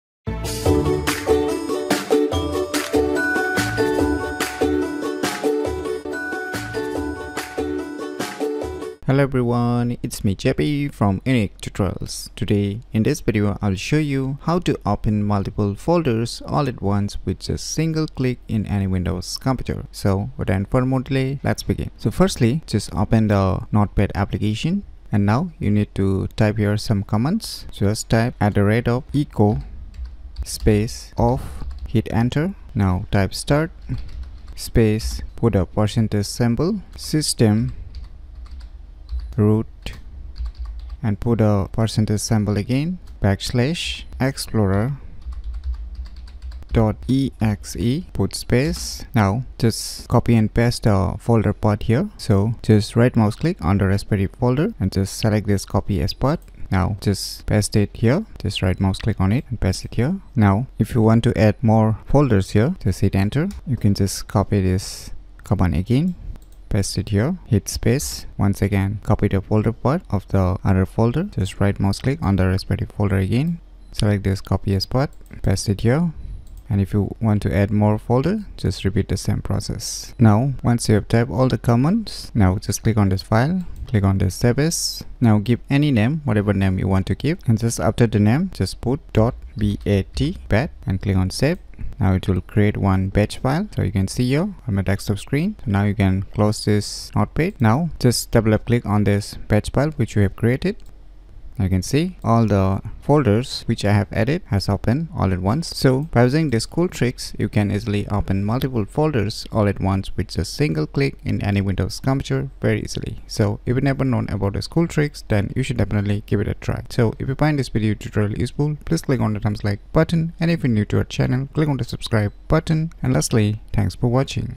How to open Multiple Folders all at once with just single click in any Windows computer ?
This video tutorial is all about How to open Multiple Folders all at once with just single click in any Windows computer.